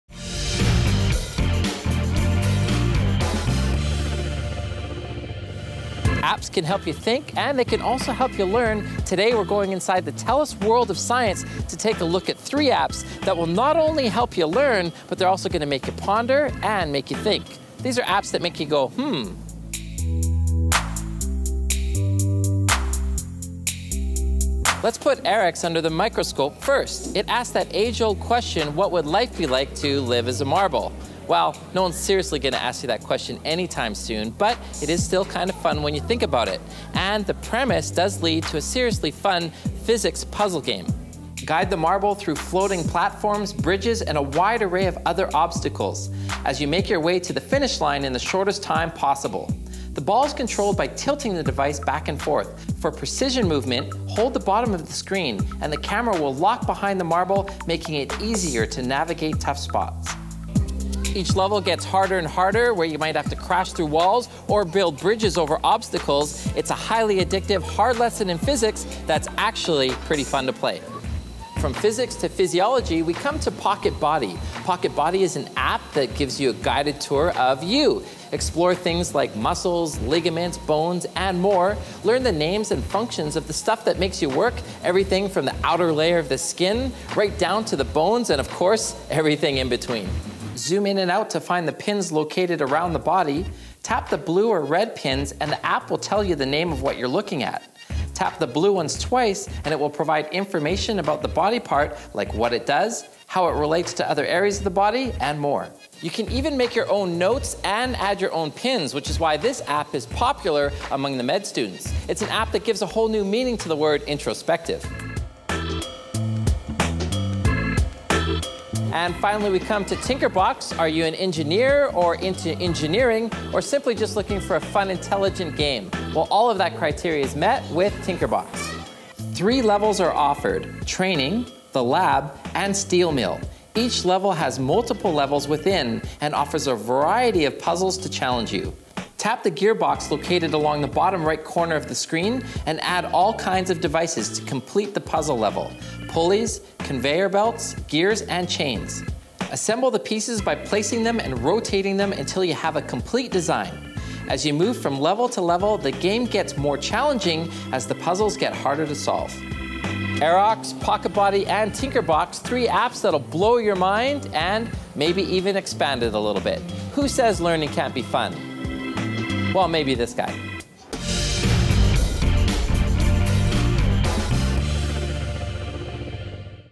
Fun Science Apps: Aerox, Pocket Body and TinkerBox
AJ looks visits the Telus World of Science to profile 3 great science apps: Aerox, Pocket Body and TinkerBox HD for the iPad.